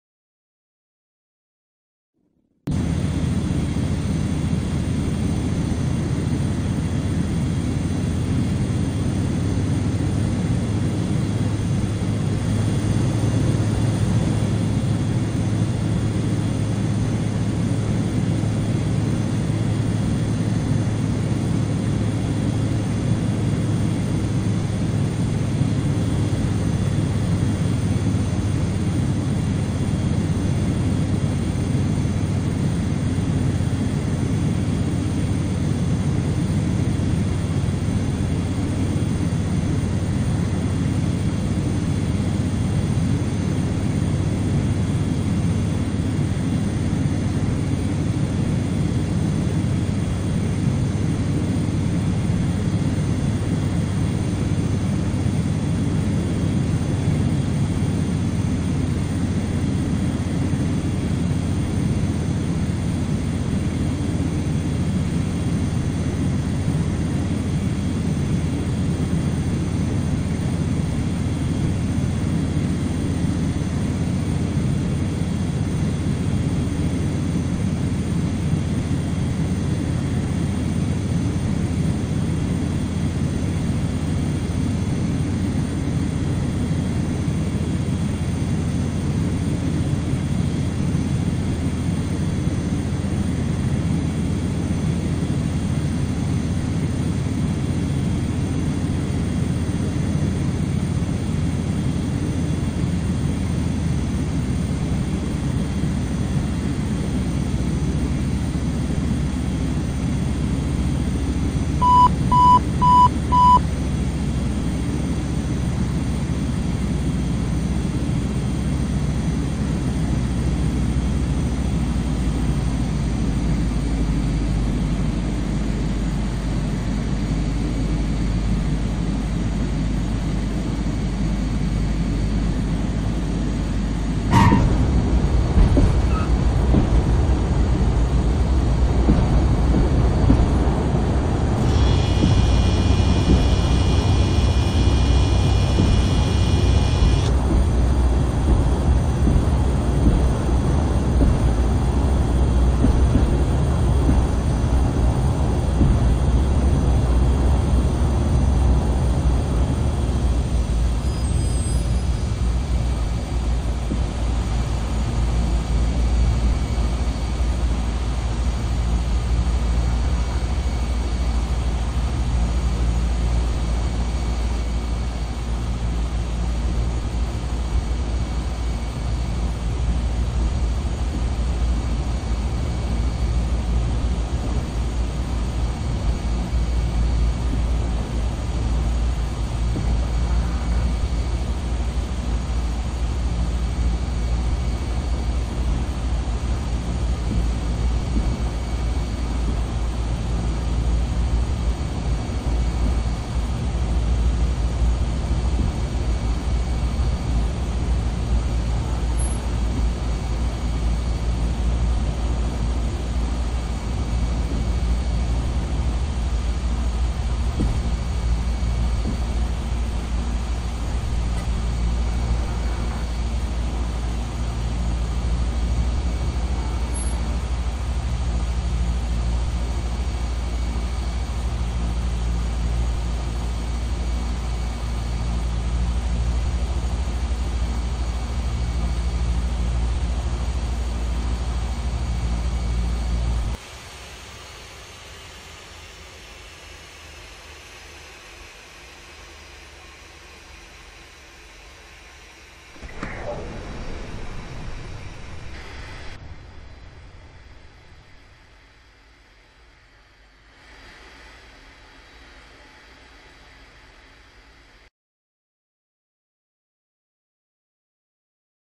Dense Fog in a C208
Dense Fog around KCRG made for an interesting approach with the Cessna 208. I saw the lights right at the minimums and decided to continue. Turned out to be an outstanding landing.
This was also the ending to my first ever (and very uneventful) VATSIM flight.

REX Worldwide Airports
REX Soft Clouds
AS16+ASCA
AccuFeel